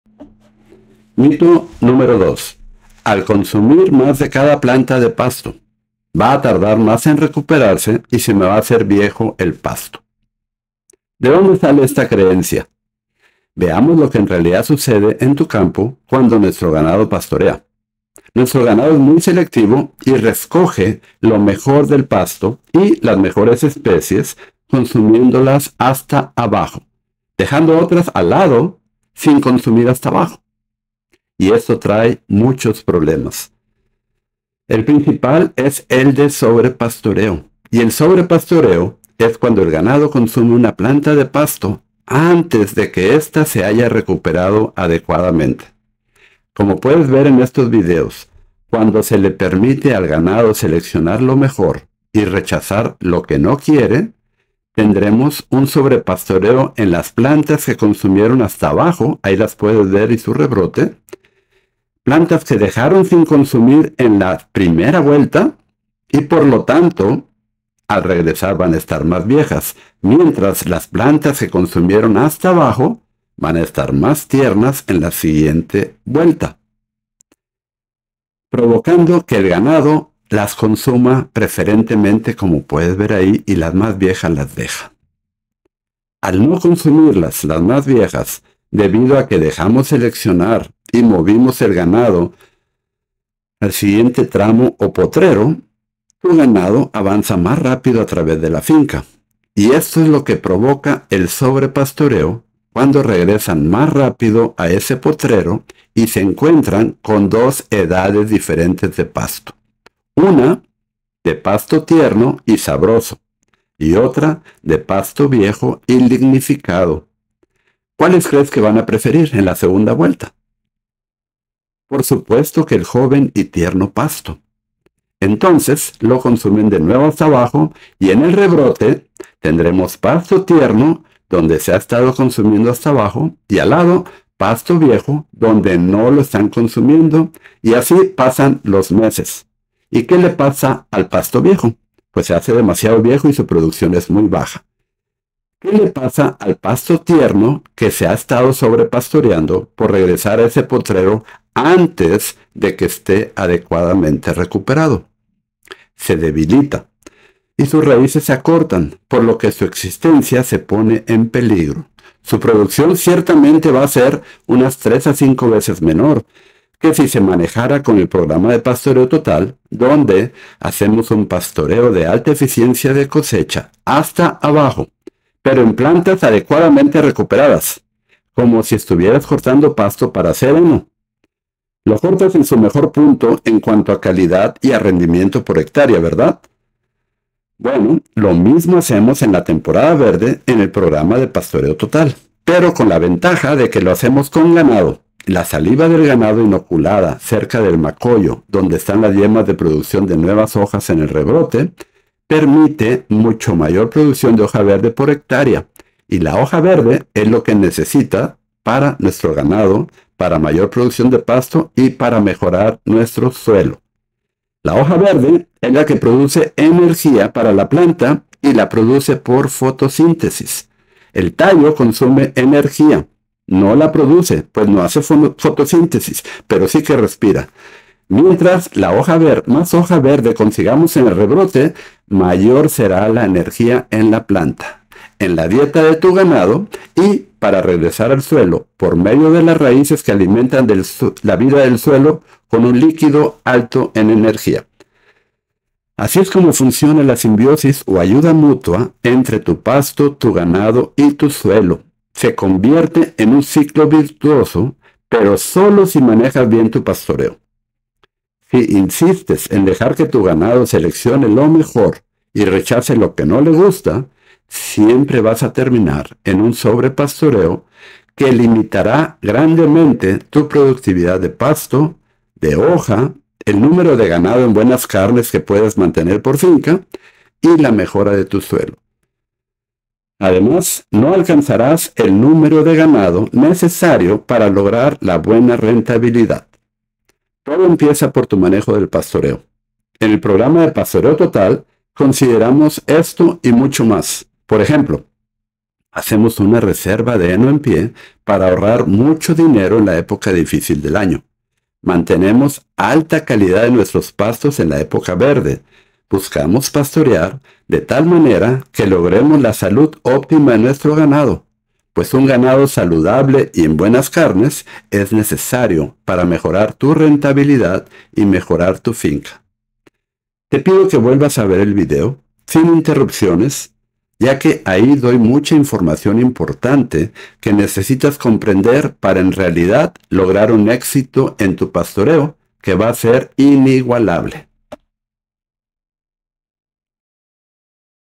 Mito #2 al cosechar mas de mi pasto lo puedo perjudicar?
Aqui explico como funcion en realidad un pastoreo, como tu ganado selecciona lo mejor y rechaza lo que no le gusta si se lo permitimos. Y que hacer para lograr la maxima produccion de pasto a bajo costo

…….FREE RESOURCES …….

…….PREMIUM RESOURCES …….

.ESPANOL? HAZ CLICK ABAJO... ACADEMIA DE PASTOREO TOTAL